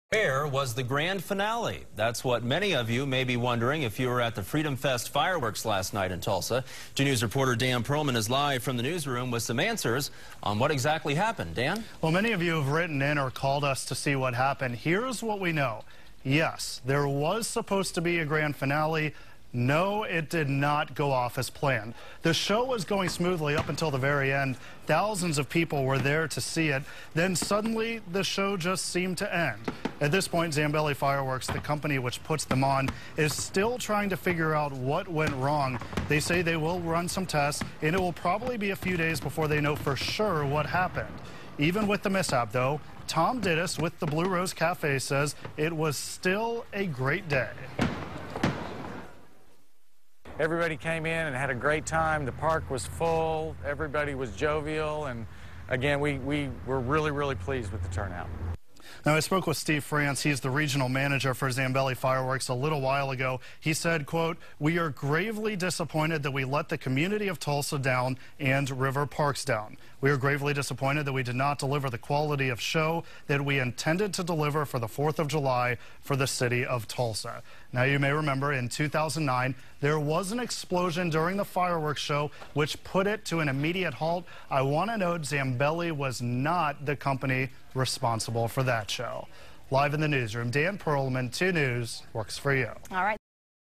Where was the grand finale?
The grand finale of the fireworks show during Freedom Fest did not go off as planned.